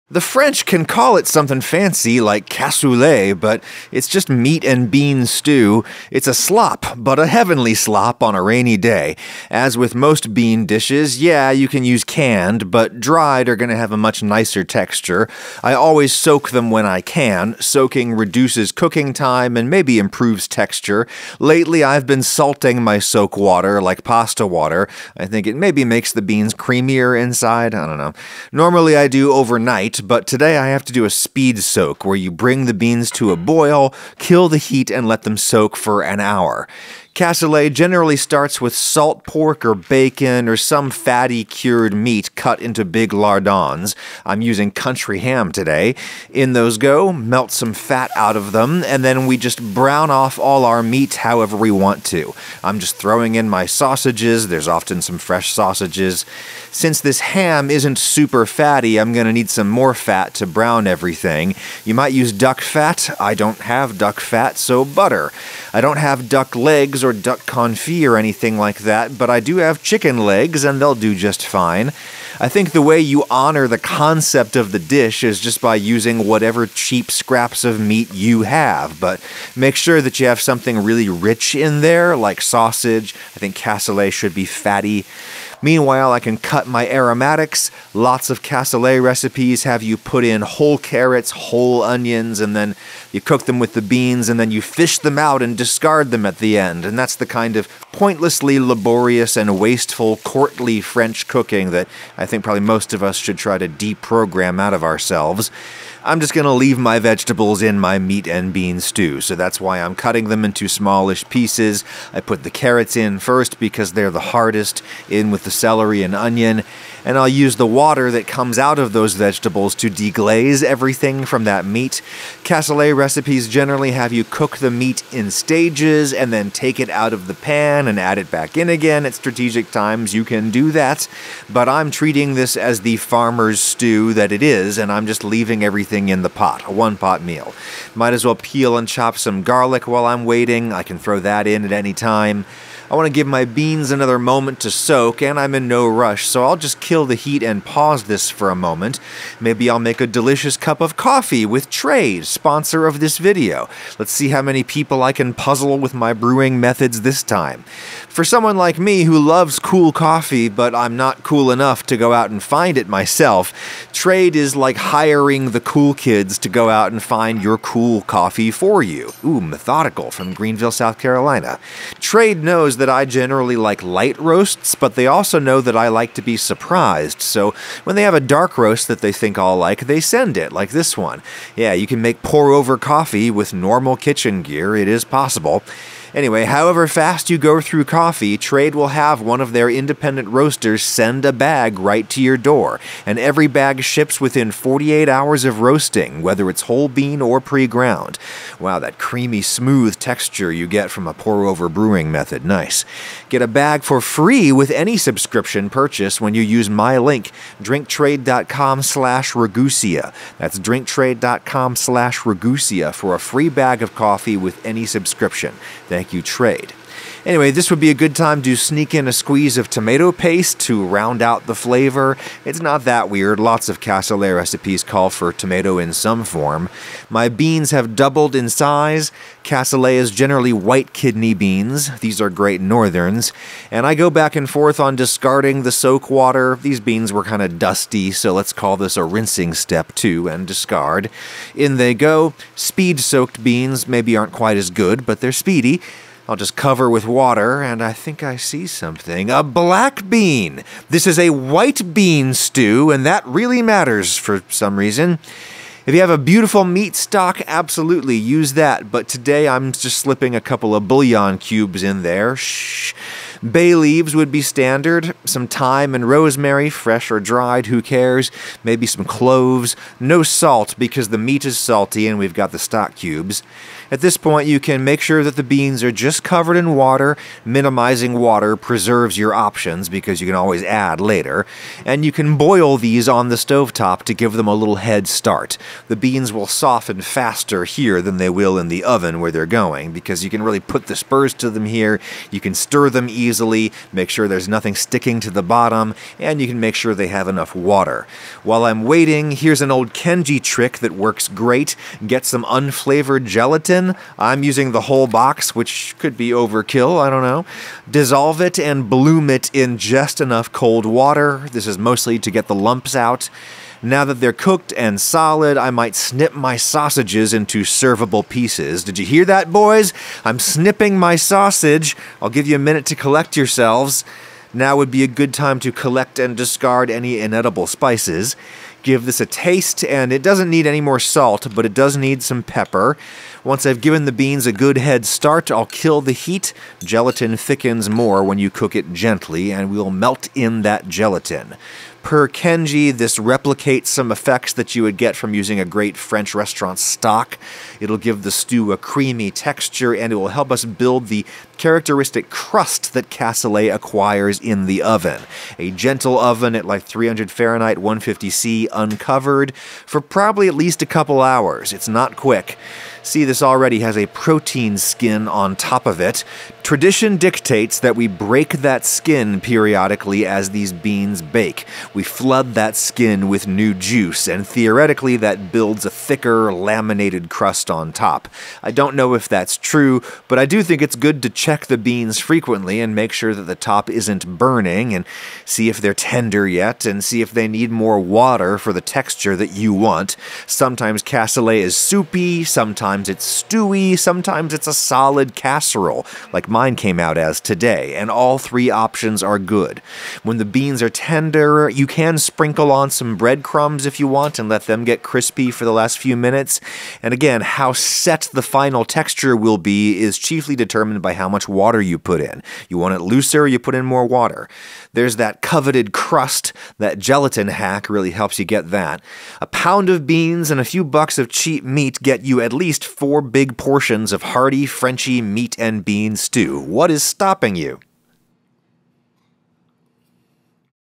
Cassoulet — Frenchy meat and bean stew
This is a method, not a recipe. It's just a beans and meat stew — improvise it!